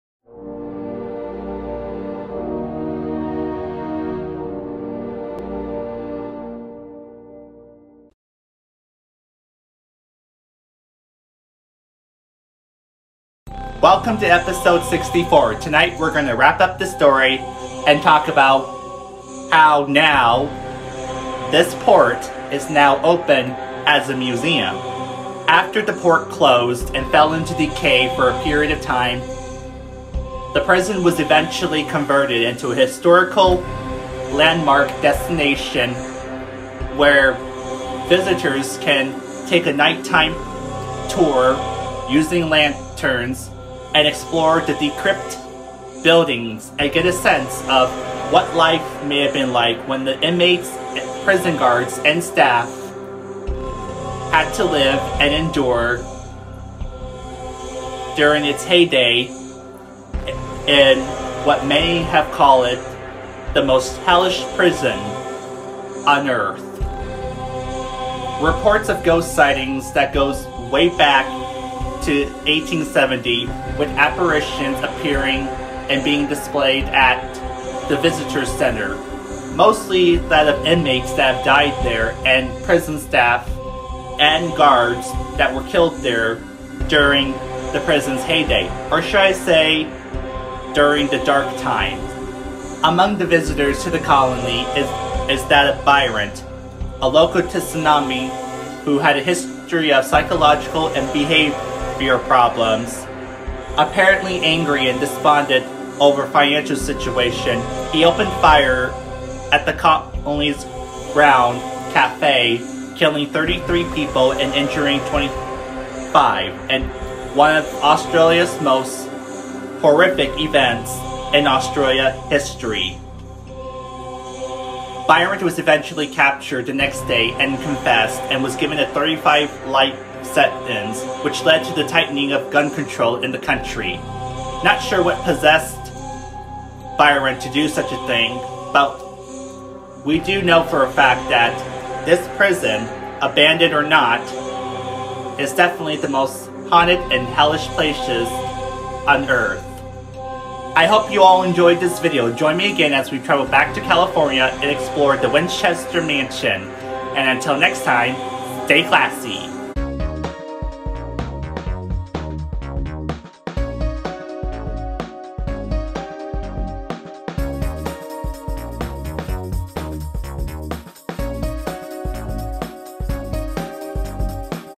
Ghost Stories Episode 64 Port Arthur: Take A Tour At The Port If You Dare
Tonight we will be wrapping up the story and talk about how now the port is open as a museum.

1740 E Stroop Rd
Kettering, OH 45429